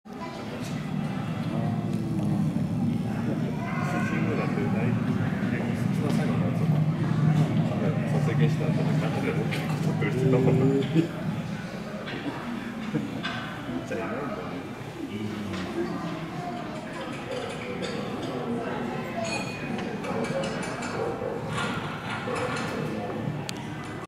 イグアノドン類 Iguanodontia：「大恐竜展」ゴビ砂漠
2013 年 10月26日（土） ～ 2014年 2月23日（日）上野・国立科学博物館で開催された「大恐竜展」ゴビ砂漠の驚異で展示されていたイグアノドン類の顎骨、下顎の化石。